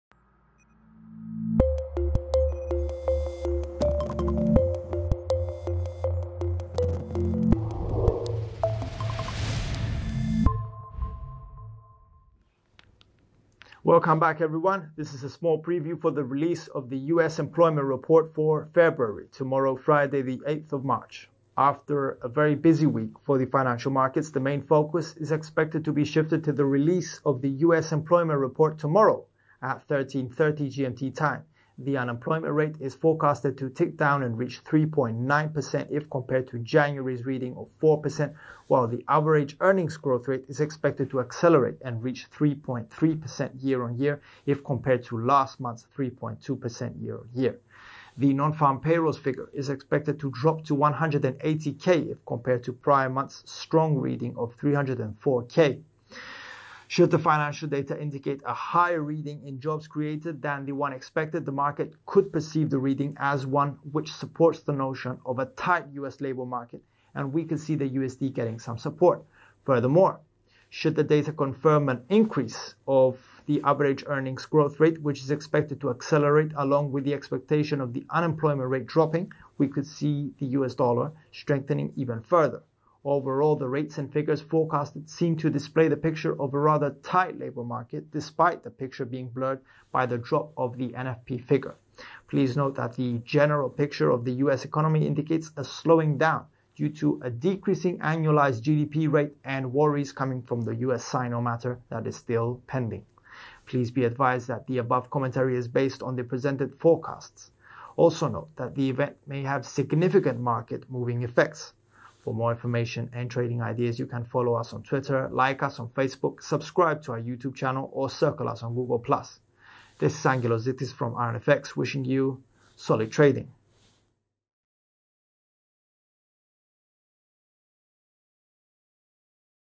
Market Focus turns to the US Employment Report on Friday | Teaser
Friday the 8th of March 2019

• US employment report for February, will be released on Friday at 13:30 (GMT).
• Non-Farm Payrolls figure: Expected at 180k, previous figure: 304k.
• Unemployment rate: Expected at 3.9%, previous reading: 4.0%.
• Average Earnings growth rate: Expected at +3.3% yoy, previous rate: +3.2% yoy.